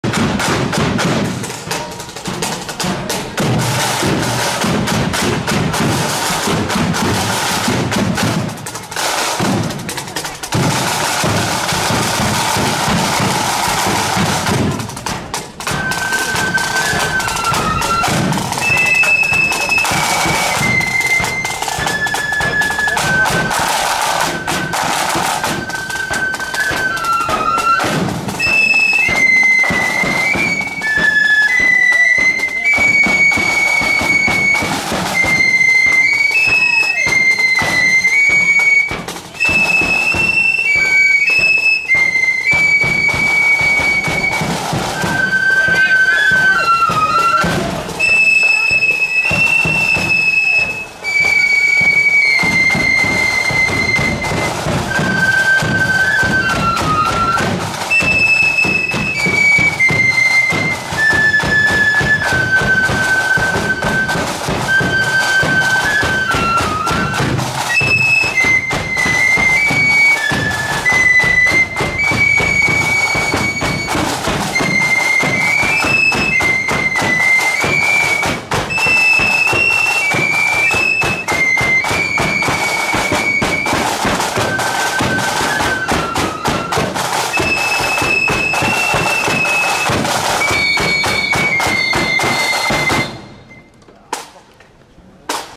Kilcluney Volunteers (No.6) @ N.P.B.'s 40th Ann. Parade ~ Newtownards ~ 23/07/22 (4K)
Kilcluney Volunteers Flute Band parading through the streets of Newtownards at Newtownards Protestant Boys Flute Band's 40th Anniversary parade held in the Co. Down town on Saturday 23/07/22.